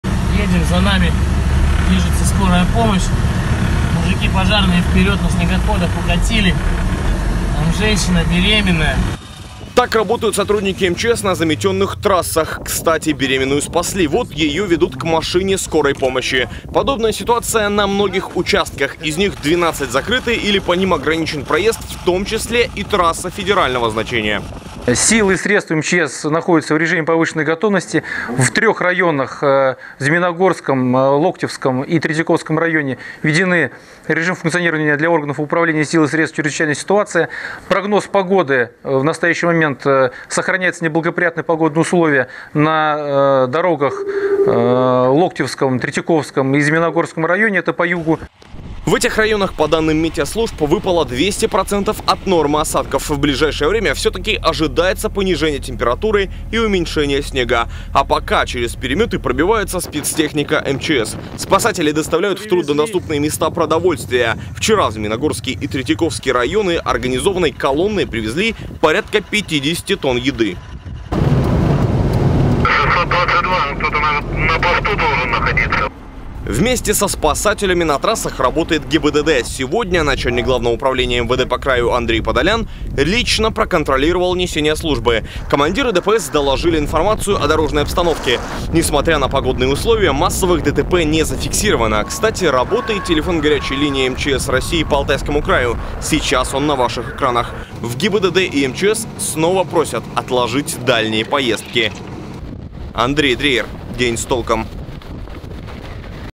Сильный ветер и метель на Алтае: в трех районах края введен режим ЧС
В Змеиногорском, Локтевском и Третьяковском районах введен режим ЧС.  Почти к ста пятидесяти населённым пунктам затруднён проезд. Как говорят метеорологи, в этих трёх районах края выпало уже 200 процентов осадков.